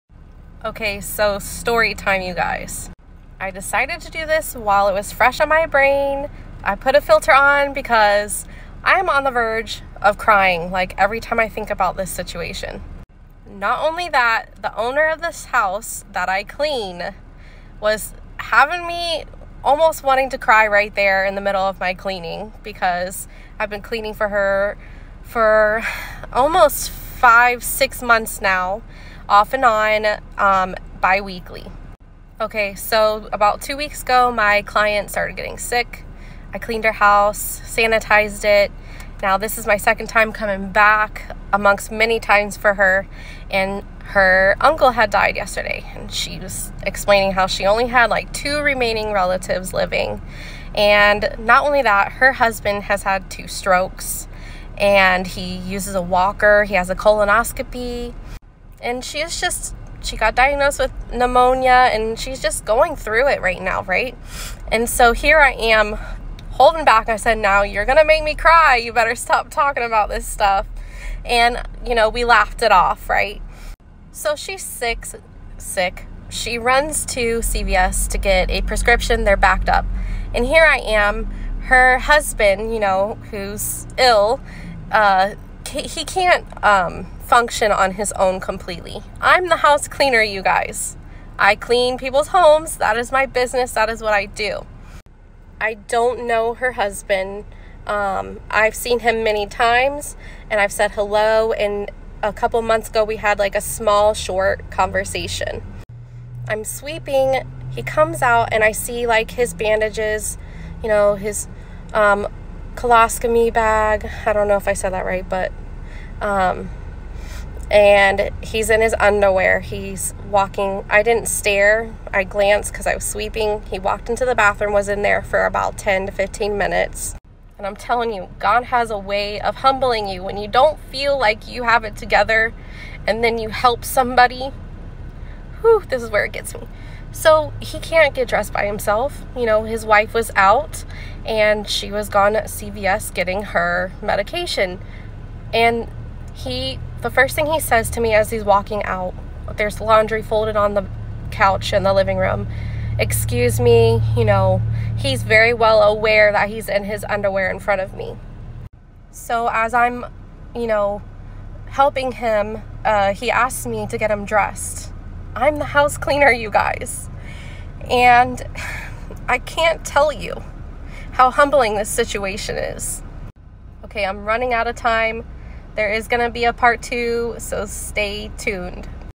The most humble situation part1
I am hired to clean many homes! But this one was the most humbling by far.